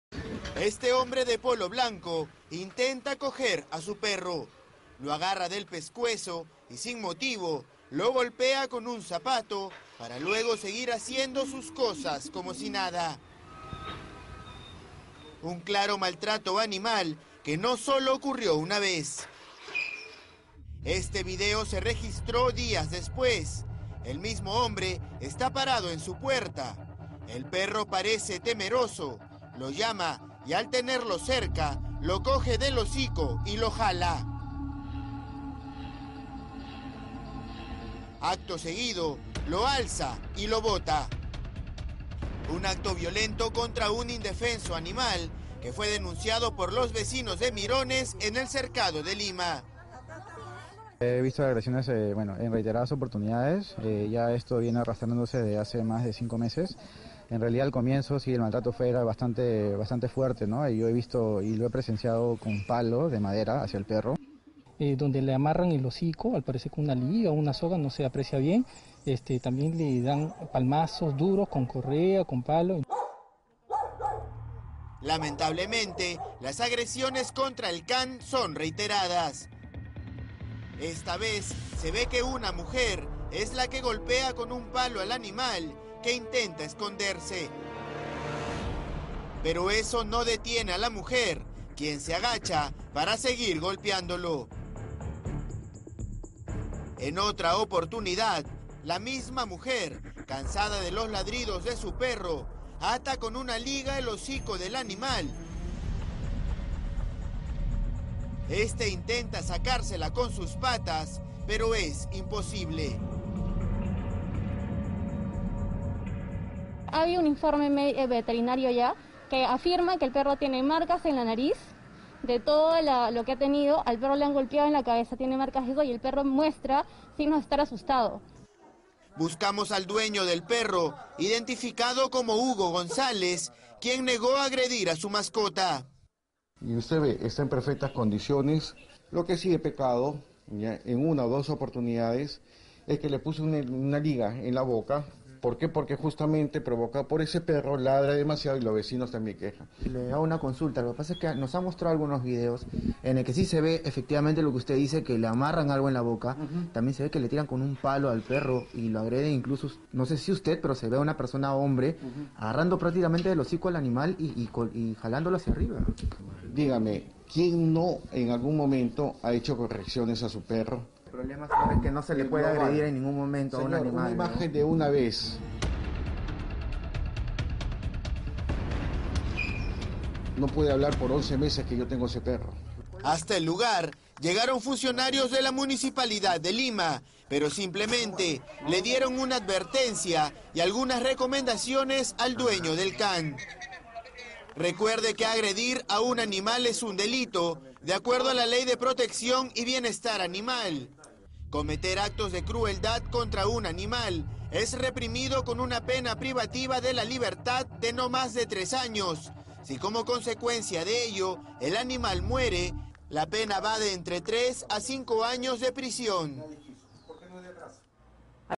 Cercado de Lima: hombre es acusado de maltratar constantemente a su perro
Los vecinos de Mirones, en el Cercado de Lima, denunciaron un nuevo caso de maltrato animal en la capital, esta vez contra un perro. Según afirman, los actos de violencia ocurrirían de manera constante.

Sin embargo, no sería la única persona que maltrata a esta mascota, pues una mujer fue captada golpeándolo con un palo y en otro momento colocándole una liga en el hocico.

Dueño negó maltrato

Ante esta denuncia, un equipo acudió donde Hugo Gonzales, dueño del can y quien negó en todo momento haber cometido actos de agresión contra este animalito. “Lo que sí he pecado es en una o dos oportunidades le puse una liga en la boca porque justamente este perro ladra demasiada, los vecinos se quejan (...). Una imagen de una vez no puede hablar por los 11 meses que tengo al perro”, dijo.

Finalmente hasta la zona acudieron funcionarios de la Municipalidad de Lima, para darle una advertencia y algunas recomendaciones.